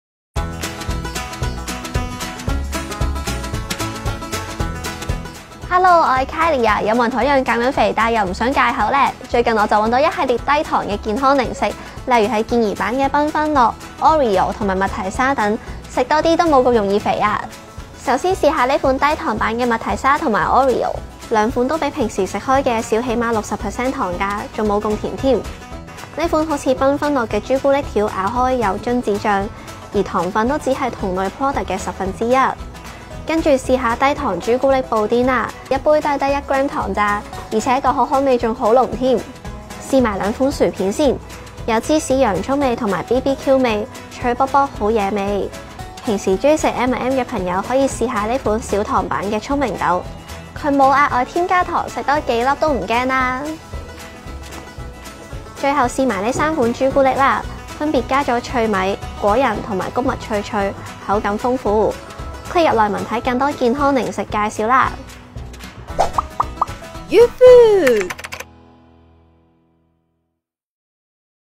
【UFood開箱】連小朋友都岩食的10款健康零食推薦！ 低糖繽紛樂／麥提莎／Oreo／朱古力布甸／芝士薯片
想減肥唔一定要戒口，不如試下呢系列低糖零食啦！
→超足料韓式燒烤聖誕到會